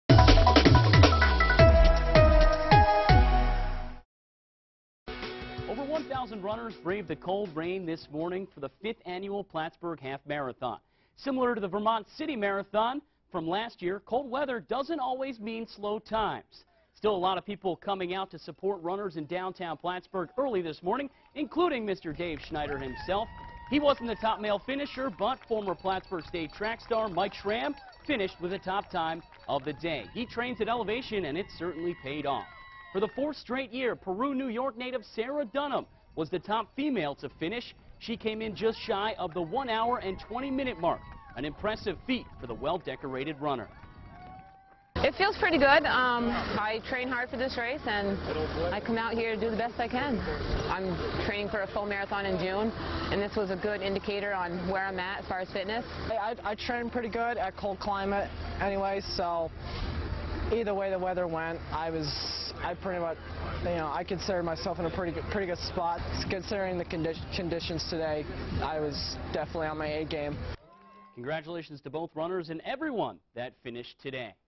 Top finishers from the Plattsburgh half marathon
Former Plattsburgh State grad and Peru native win men's and women's races